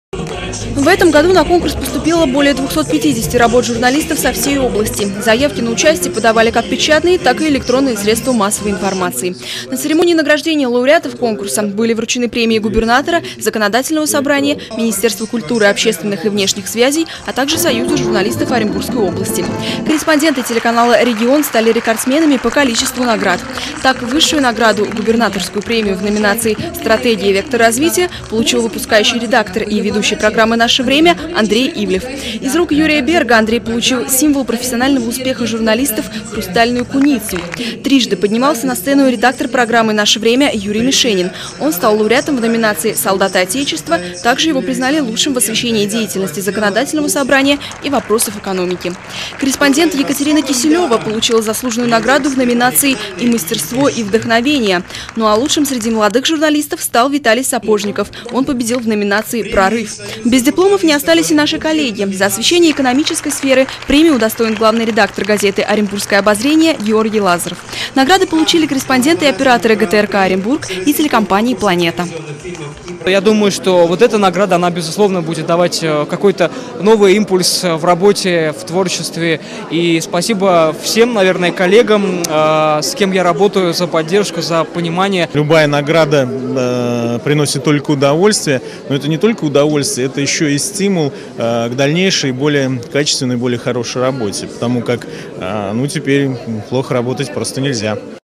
Лучшие в журналистике 29-01-12
Отдельная программа  медиа-форума — награждение работников СМИ, победителей региональных конкурсов. В этом году на конкурс поступило более 250 работ журналистов со всей области. Заявки на участие подавали как печатные, так и электронные средства массовой информации. На церемонии награждения лауреатов конкурса были вручены премии губернатора, Законодательного Собрания, министерства культуры, общественных и внешних связей, а также  Союза журналистов Оренбургской области. Корреспонденты телеканала «Регион» стали рекордсменами по количеству наград. Так, высшую награду, губернаторскую премию в номинации «Стратегия — вектор развития» получил выпускающий редактор и ведущий программы «Наше время» Андрей Ивлев. Из рук Юрия Берга Андрей получил символ профессионального успеха журналистов — хрустальную куницу. Трижды поднимался на сцену редактор программы «Наше Время» Юрий Мишенин. Он стал лауреатом в номинации «Солдаты Отечества», также его признали лучшим в освещении деятельности Законодательного Собрания и вопросов экономики. Корреспондент Екатерина Киселева получила заслуженную награду в номинации «И мастерство, и вдохновение». Ну а лучшим среди молодых журналистов стал Виталий Сапожников. он победил в номинации «Прорыв».  без дипломов не остались и наши коллеги. За освещение экономической сферы премии удостоен главный редактор газеты «Оренбургское обозрение» Георгий Лазаров. Награды получили корреспонденты и операторы ГТРК Оренбург и телекомпании «Планета».